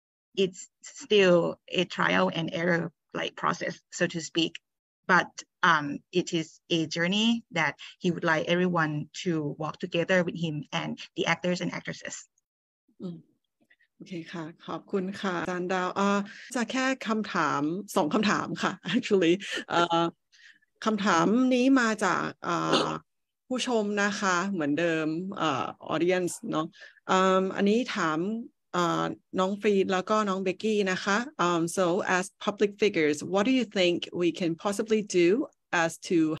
A boss like Saint 🥺🥰
Yesterday Saint was a panelist with Freen and Becky discussing issues regarding Y series (BL/GL) at QueerFanTHasia.

(Centre for Cultural Studies, CUHK)
✨ Original clip: